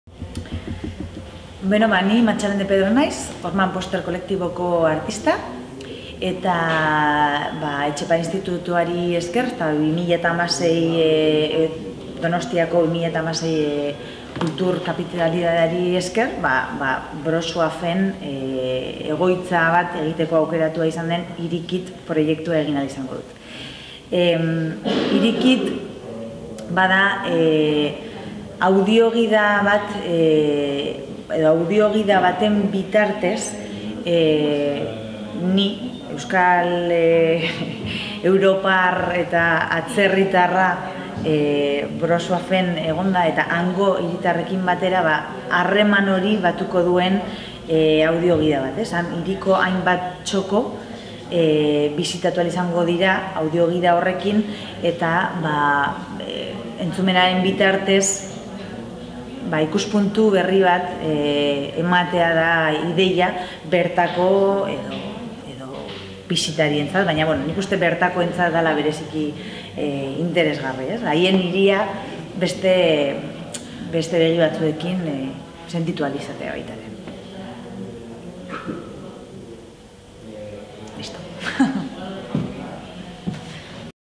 Matxalen de Pedro: "Hiri kit". Wroclaw - Etxepare 2016. Donostia 2016 / San Sebastián 2016
[EUS] HIRI-KIT-ek hiriko paisaia urbanoan murgiltzeko esperientzia eskaintzen du, Wroclaw-eko biztanleen eta bisitari baten arteko elkarbizitzaren bitartez, hiria topaleku, memoria, fikzio bilakatuz. Jarraibide batzuk dituen mapa batekin eta batez ere audio-gida batek lagundurik, bertako biztanleak hiria zeharkatuko du beste ahots eta begirada batez hornitua.

[CAS] HIRI-KIT ofrece la oportunidad de sumergirse en el paisaje urbano a través de la convivencia entre los y las habitantes de Wroclaw y una visitante, convirtiendo la ciudad en punto de encuentro, memoria, en ficción. Por medio de un mapa con algunas indicaciones  y sobre todo con ayuda de una audio-guía, los ciudadanos y ciudadanas de Wroclaw podrá recorrer su ciudad con ayuda de otra voz y de otra mirada.